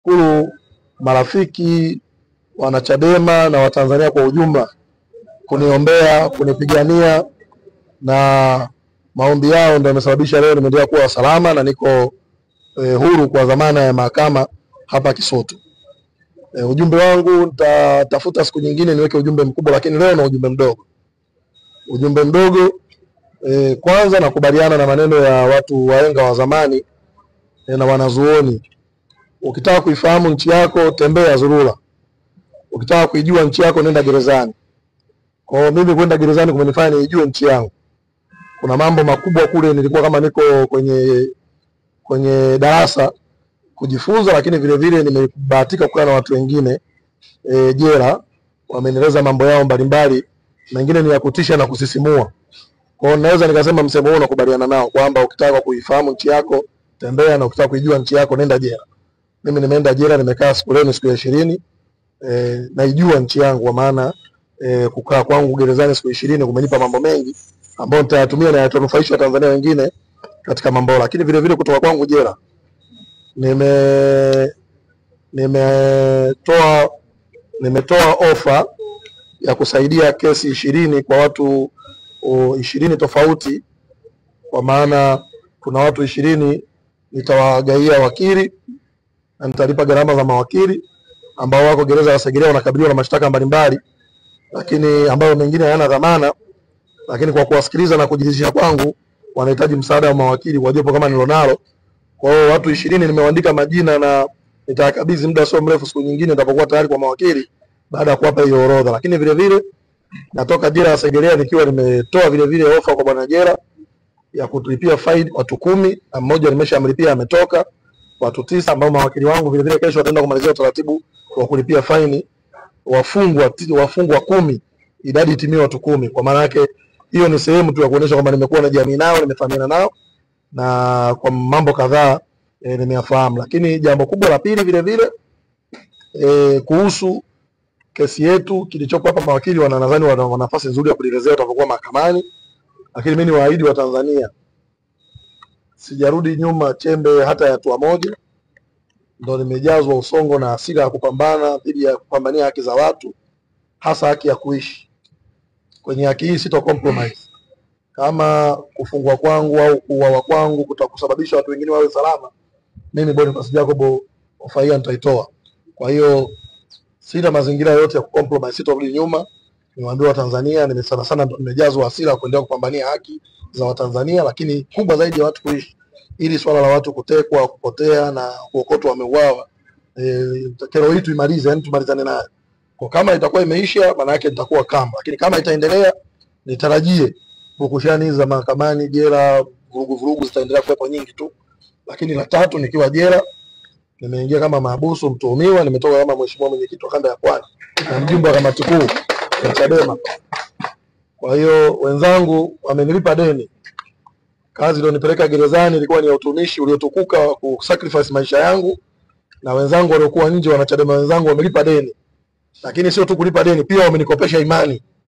KAULI YA KWANZA YA BONIFACE JACOB BAADA YA KUTOKA JELA
tanzania bonifacejacob Mwanachama wa CHADEMA, Boniface Jacob (Boni Yai) amesema ametoa ofa ya kuwalipia faini Wafungwa 10 waliopo katika Gereza la Segerea ili waachiwe huru

Amesema hayo baada ya kuachiwa kwa dhamana katika Mahakama ya Hakimu Mkazi Kisutu, leo Oktoba 7, 2024